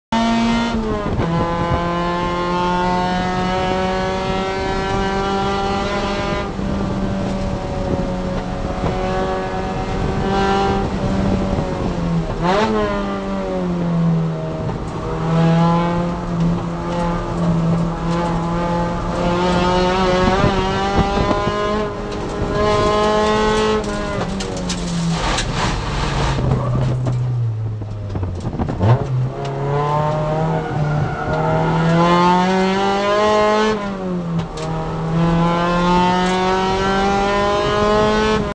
How not to pass at a track day.
Pointed by, but pinched off track.